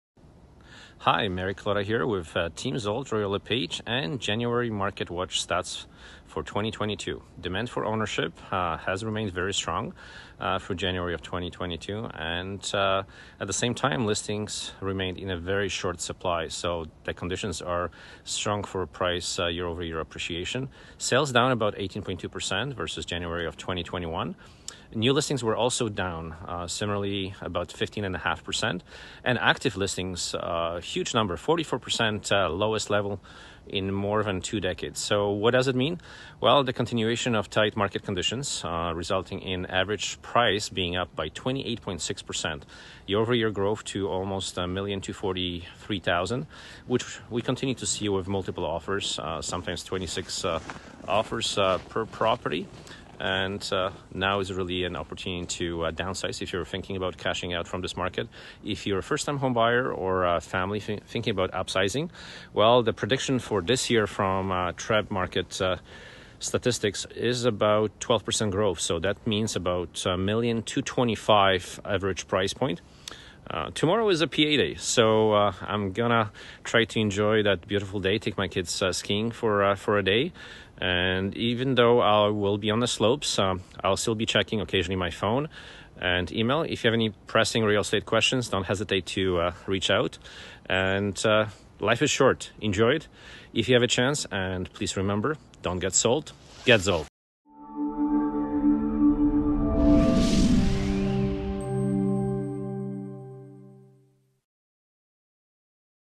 January 2022 Real Estate Market Update
There were 5,636 sales reported through the TRREB MLS® System in January 2022 – down by 18.2 per cent compared to 6,888 sales in January 2021. While sales were down substantially compared last year’s record result, the January 2022 result was the second best in history for the month. This result is in line with TRREB’s forecast for a strong sales result in 2022, but off the 2021 record.

New listings were down by a similar annual rate (-15.5 per cent) as sales. Because sales and new listings moved in relative lock step, active listings at the end of January amounted to 4,140 – down by 44 per cent to the lowest level in more than two decades.